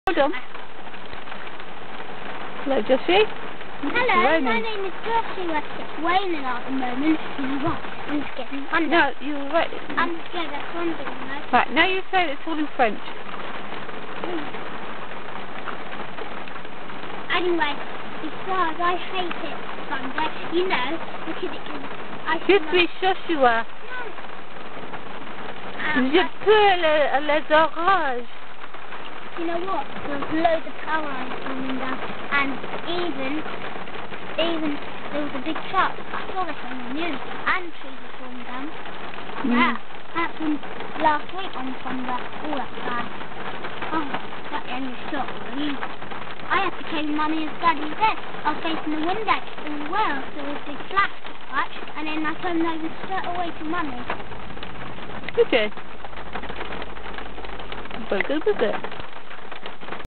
Joshy chatting about the weather!
My 8 year old son scared of possible thunderstorm and heavy rain!  bless him so cute!